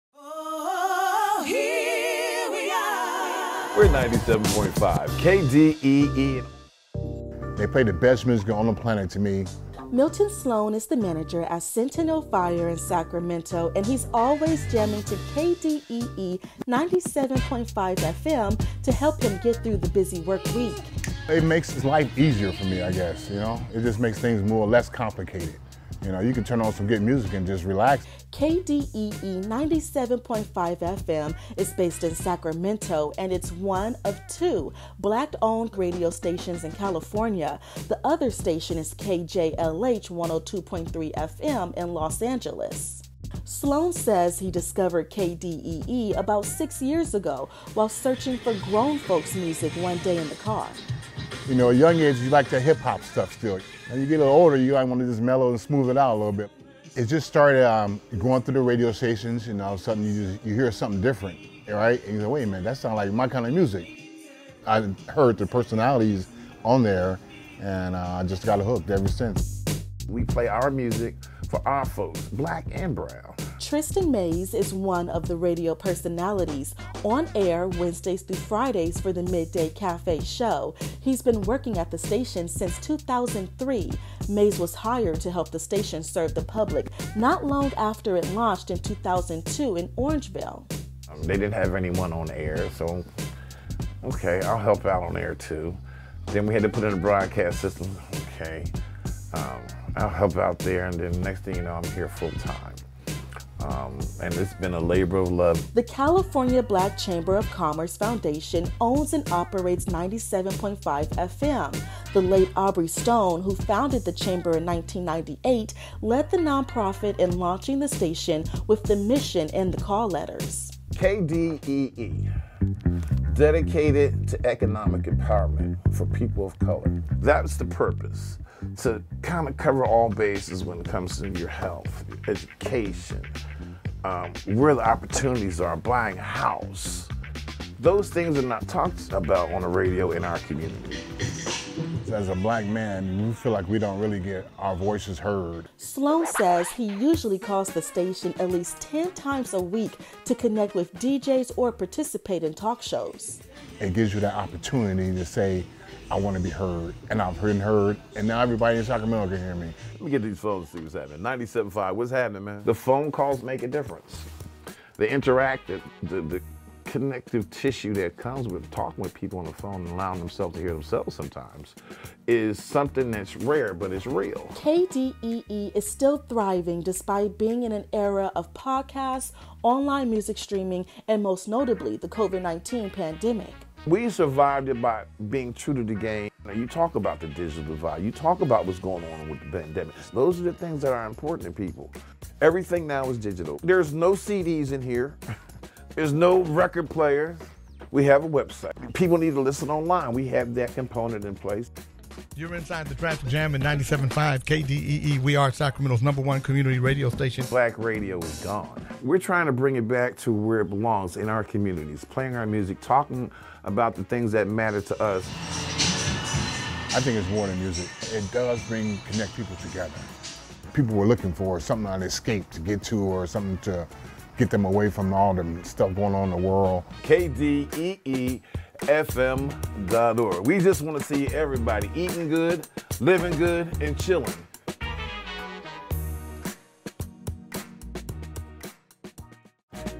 KDEE: History of Sacramento's only Black-owned radio station
In California, there are only two Black-owned and operated radio stations. One of them is KDEE 97.5 FM radio in Sacramento. The radio station provides more than just music-- it also informs Black people about news, community events and offers public service information.

Photo Credit: ABC10/KXTV